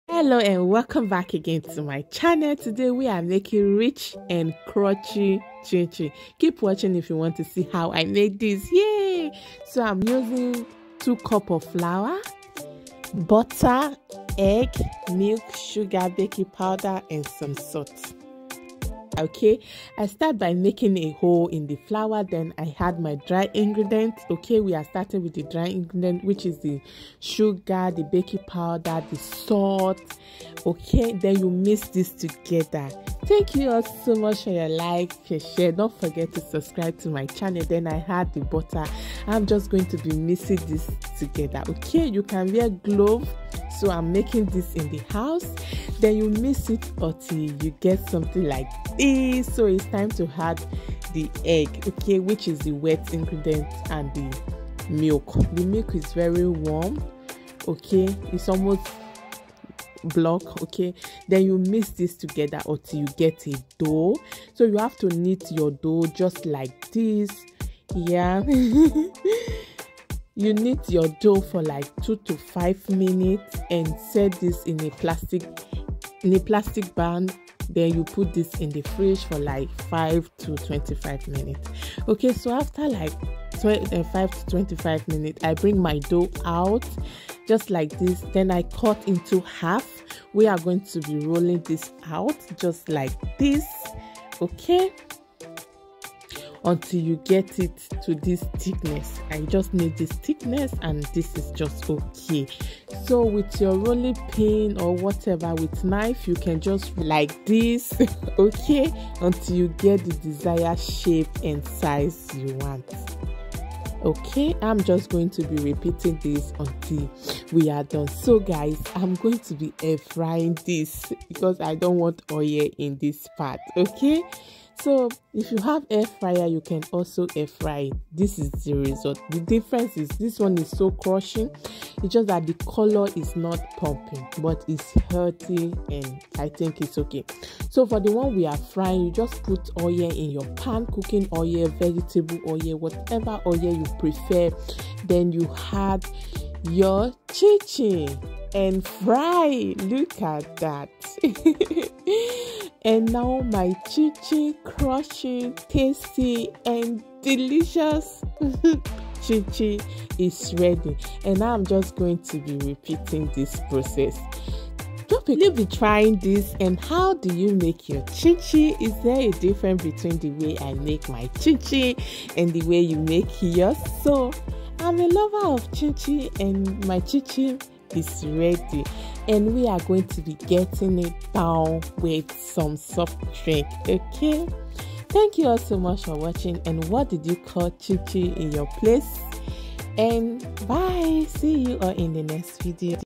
how to make crushing and rich chinchin ...stey by step method/Nigeria tasty chinchin
growing up in Nigeria as a young lady;;chinchin was one of my favorite snacks yess...and this right here remind me of home and it's beautiful food this chinchin is so rich and crushing